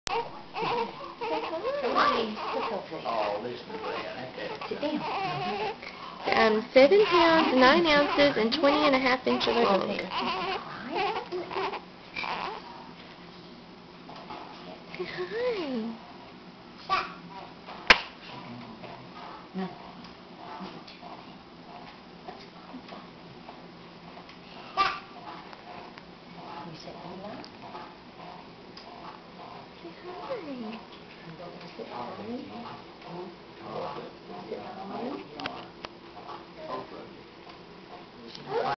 Elijah Eben Devine Birthday
Eli is only a few hours old in the video!  He was born 8-22-08 and is such a pretty baby!